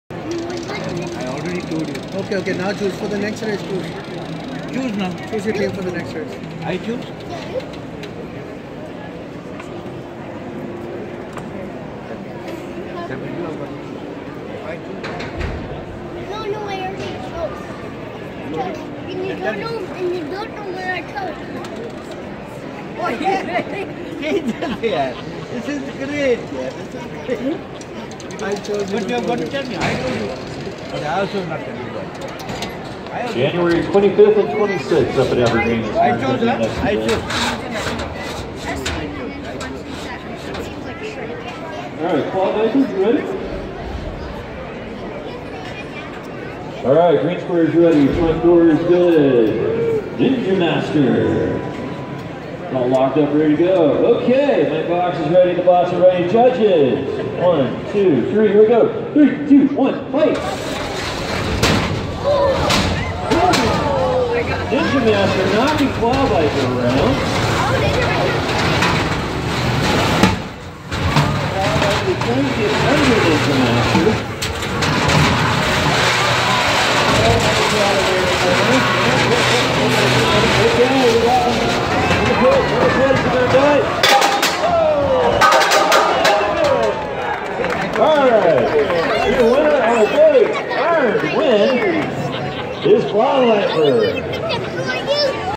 Battle of the Bots Seattle Center
During the 2019 Robots Convention at Seattle Center, competitors battle each other for prize and glory.  This is the 5 pounds division.